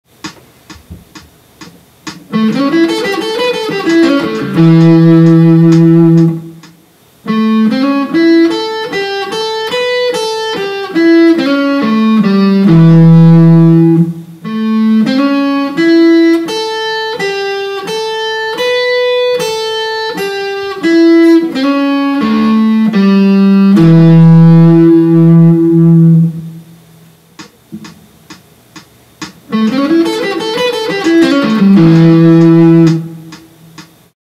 Guitar Instructor Lick of the Week 3/1/2011 - Elmore James turnaround
Electric Delta blues turnaround a la Elmore James.